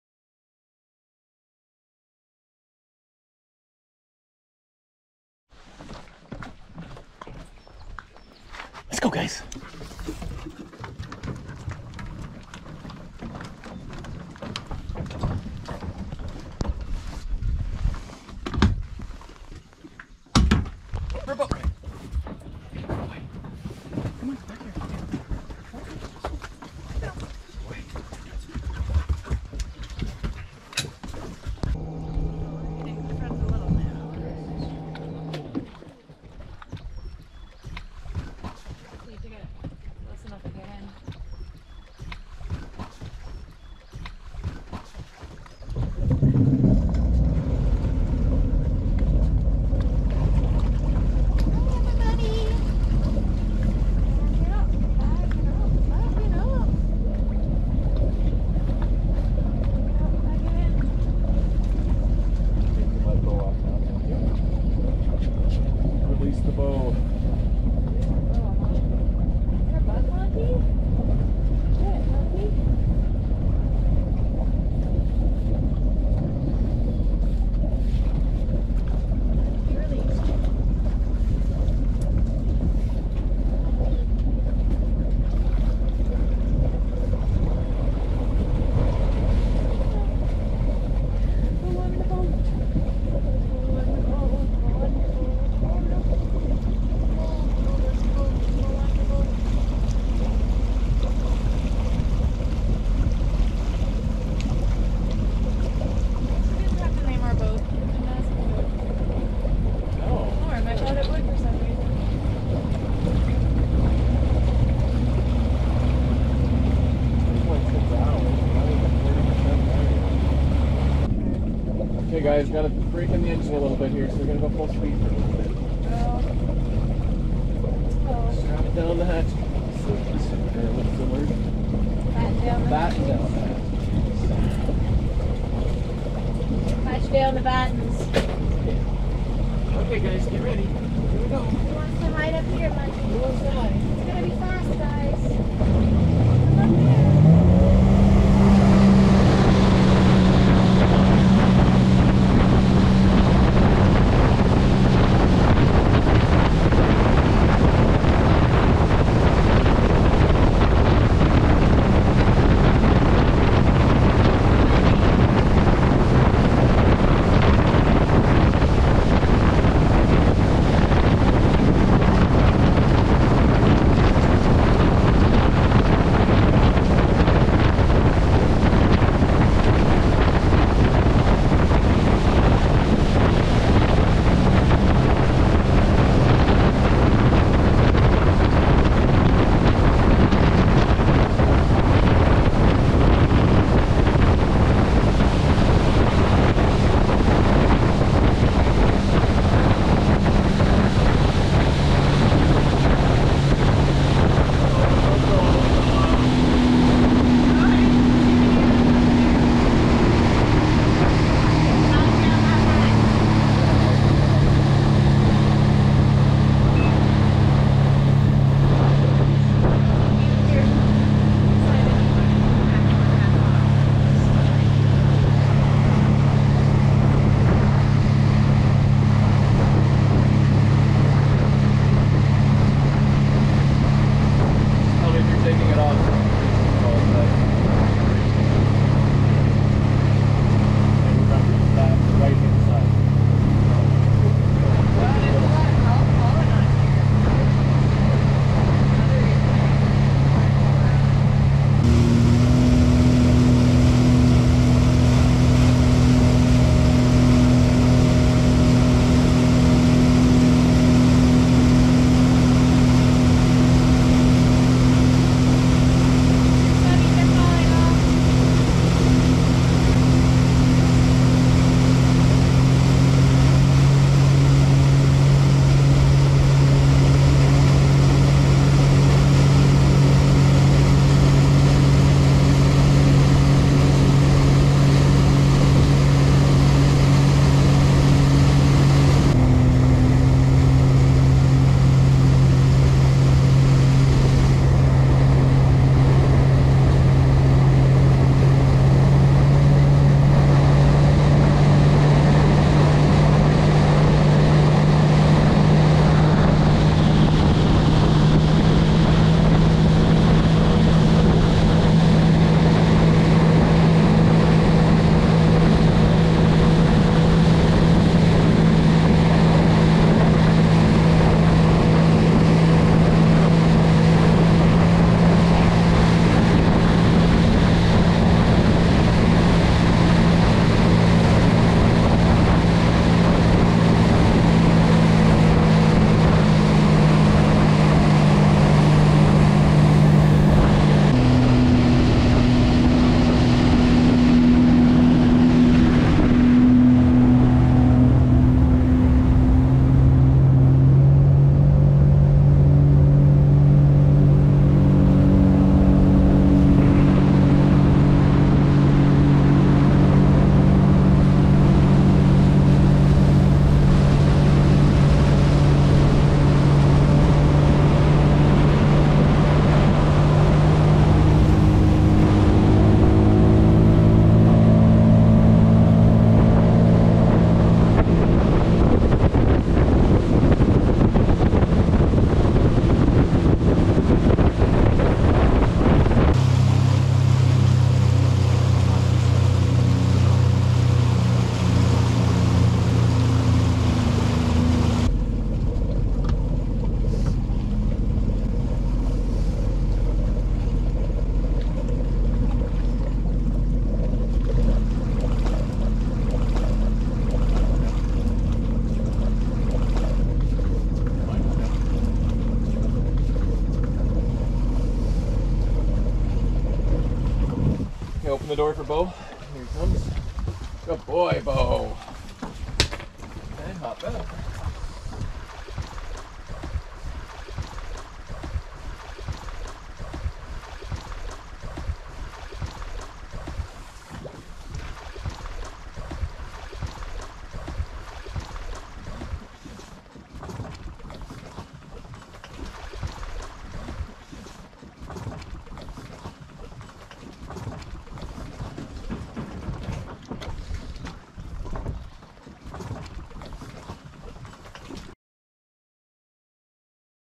2023 Sea Doo Switch Sport - Break In Period
2023 Sea Doo Switch - 18 foot, 230hp.  4.7 hours into the break-in period, which is 10 hours by the book.  I'll have to figure out a better position for the GoPro!

Sea Doo
Sea Doo Switch